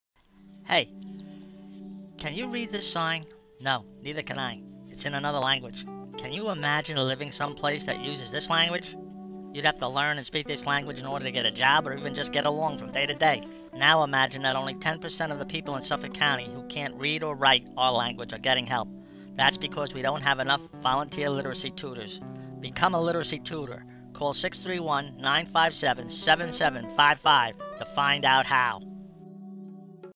LITERACY NOW @ LML: DANGER!
Only 10 percent of those in Suffolk County who can't read or write are getting help because of a lack of trained literacy tutors. Become a volunteer literacy tutor.